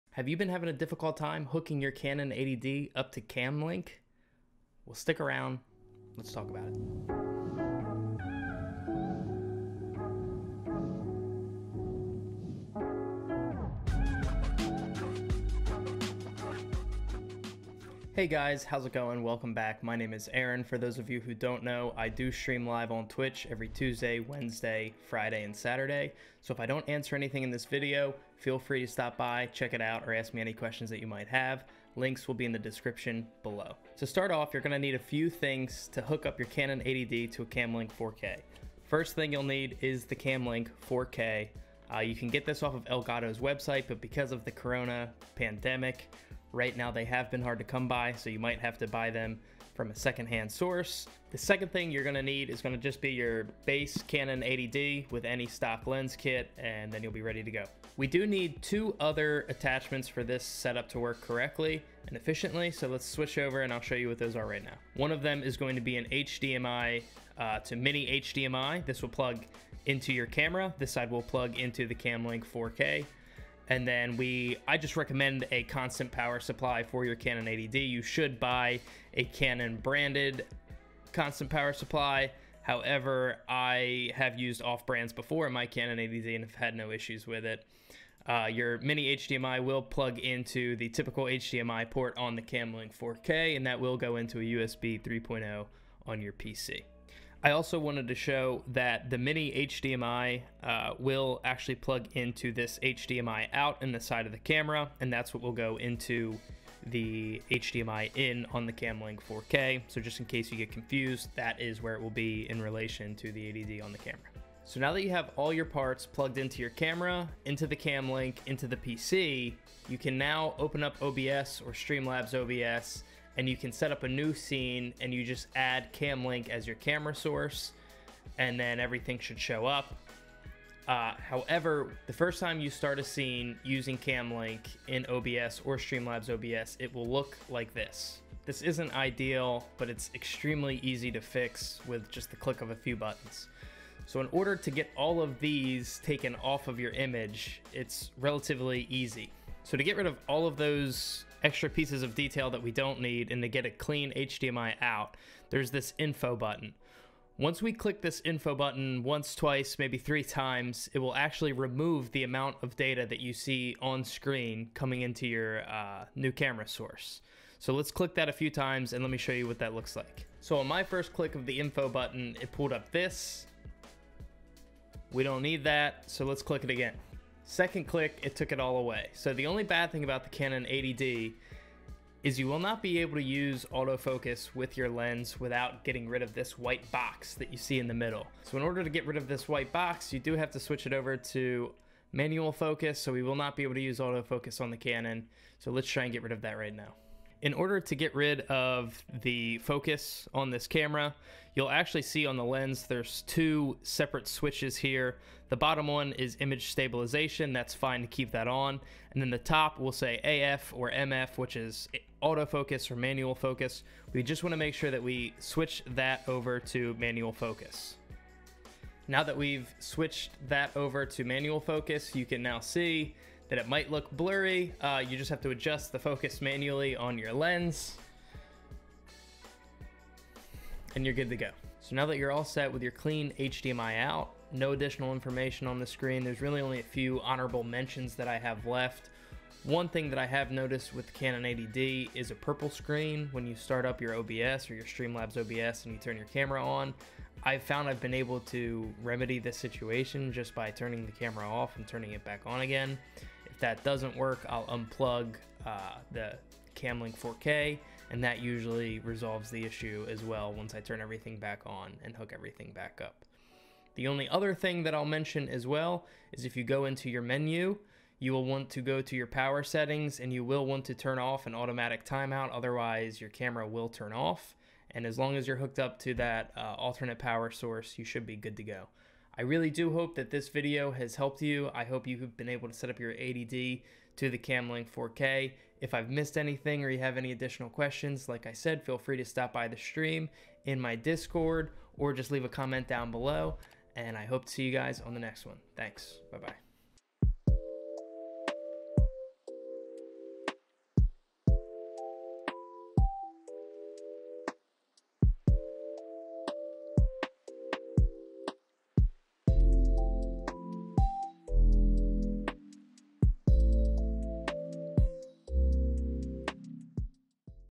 Cam Link Setup with a Canon 80D (DSLR) - Clean HDMI Out | StreamDreams
In this video on the StreamDreams playlist, I review how to setup the Canon 80D to Cam Link 4K to get a Clean HDMI Out! If you don't have a Canon 80D, but have a similar model or DSLR, you can check if your camera is compatible by visiting Elgato's Website (linked below). Some steps or equipment needed may be different on different model cameras. If you want to check on any specific step feel free to use the timestamp shortcuts below. I have also linked some equipment mentioned in the video (I am not sponsored or paid for any advice or equipment purchases made). If you have any questions please leave them in the comments, or feel free to check out my twitch and discord for more help! Happy Streaming 😎

► HDMI to Mini HDMI
► Continuous Power Source (for Canon 80D)

⏩Video Shortcuts ⏩

00:00 - Intro
00:34 - Equipment Needed
01:28 - Where the HDMI goes
02:00 - How to Create Cam Link Source in SLOBS
02:18 - How to Get a Clean HDMI Out
03:00 - Removing Auto Focus on Canon Lense
04:00 - Other Things to Note/Troubleshoot (Purple Screen)
04:24 - Disabling Auto Power Off in Camera
04:39 - Outro